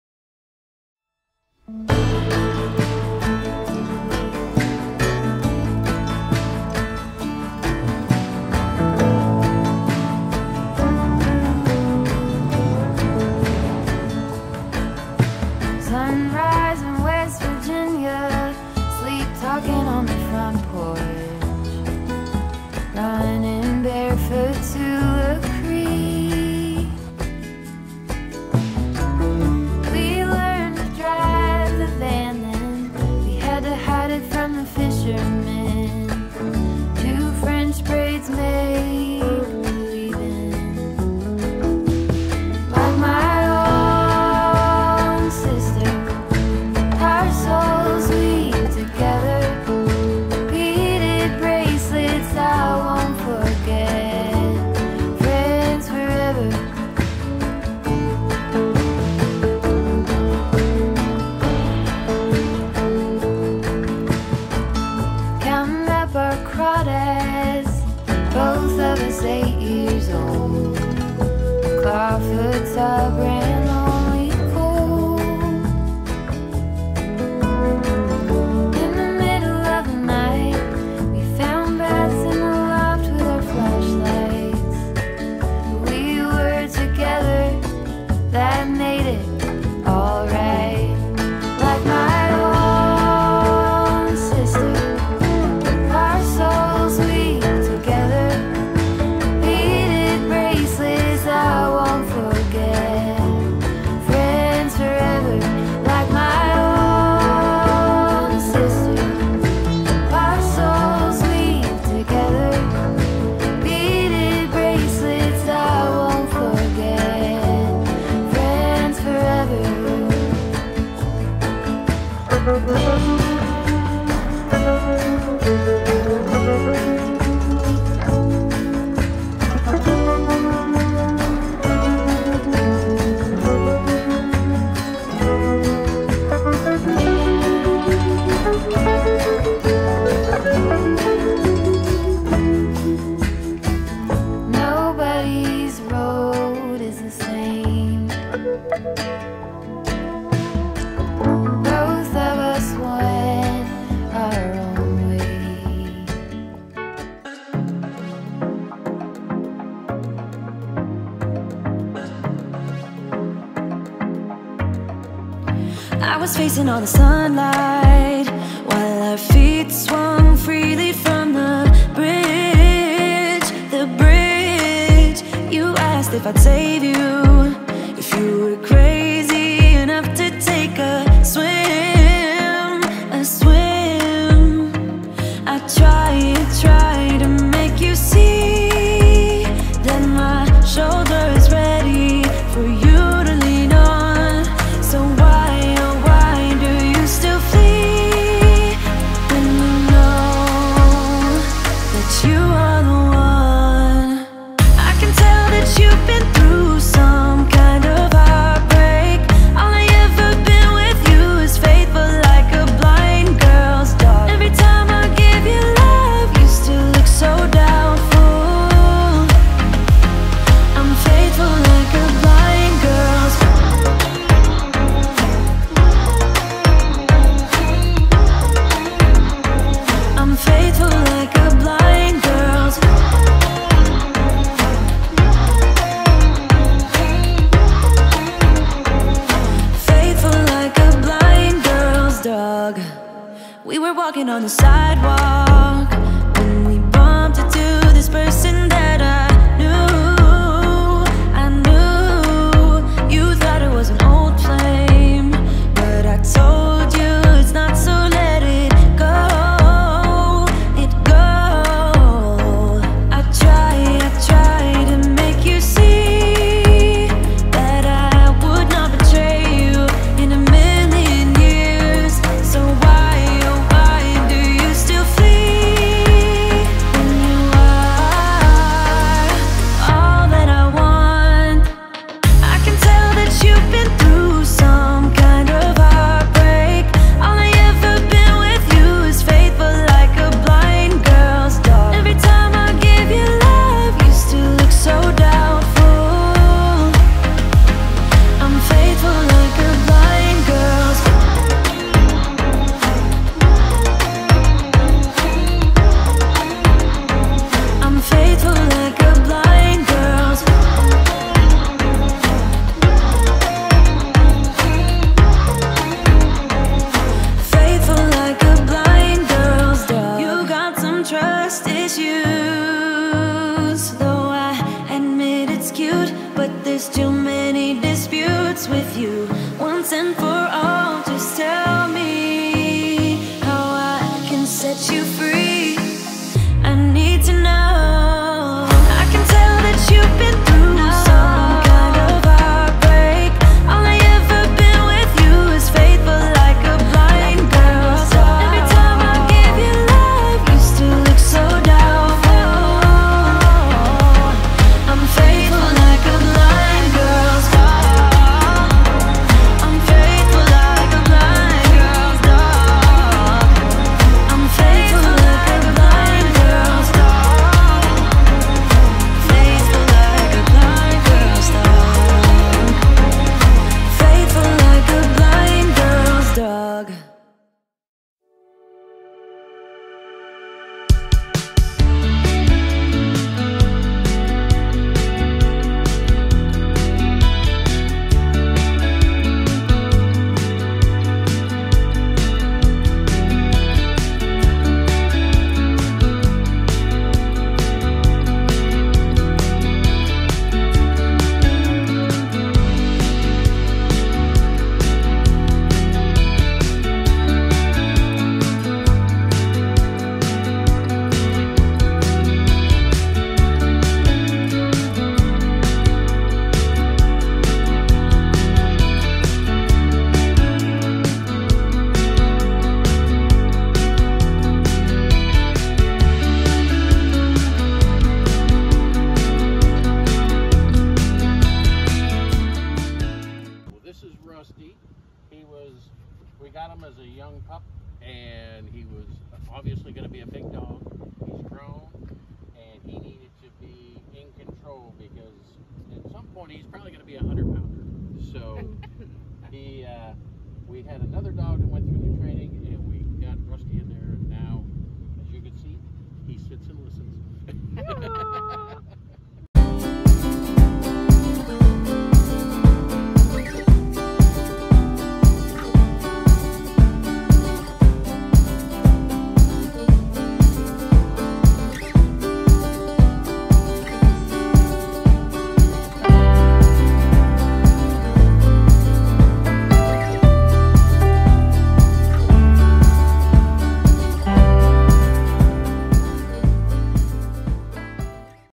The Complete K9 - 6 Month Old Rhodesian Ridgeback Goes To Obedience School!
Rusty is a 6 month old Rhodesian Ridgeback who did not realize just how big he is and still wanted to be carried for car rides thinking he was still so little. He used to pull his parents on the leash anywhere he wanted until he joined our pack with our Two Week Board & Train program. Rusty learned good manners to take home and confidence that's going to last forever! So proud of you buddy!

- 1st Lesson Money Back Guarantee on ALL First Classes!
- Lifetime Support Guarantee on Board and Train Sessions!
- 0% Interest, No Credit Check Payment Plans Available!